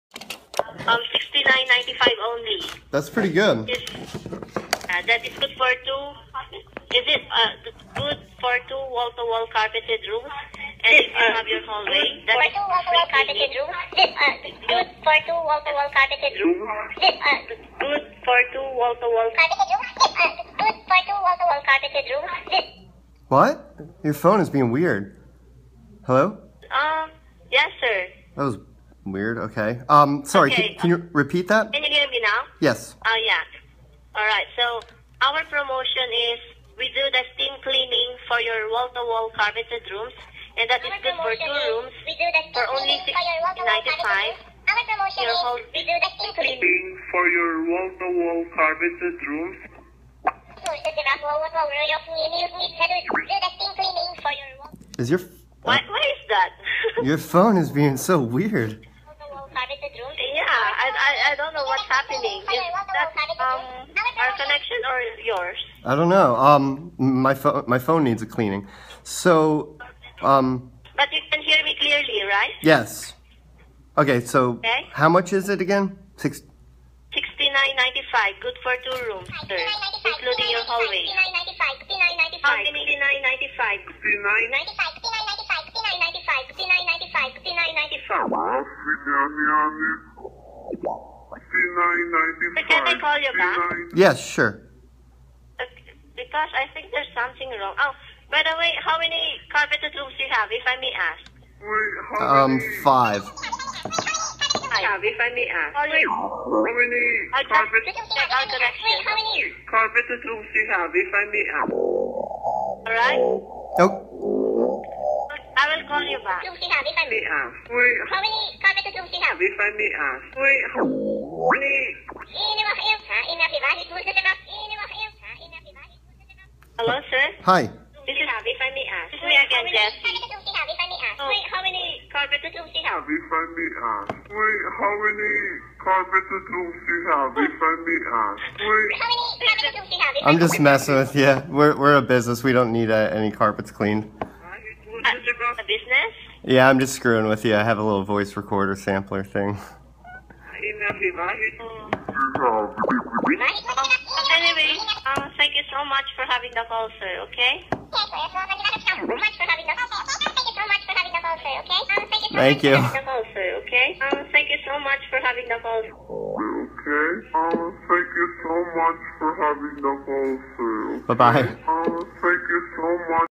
Trollicitor - Carpet Rompler - Solicitor Prank Phone Calls
Trolling solicitors to save the world.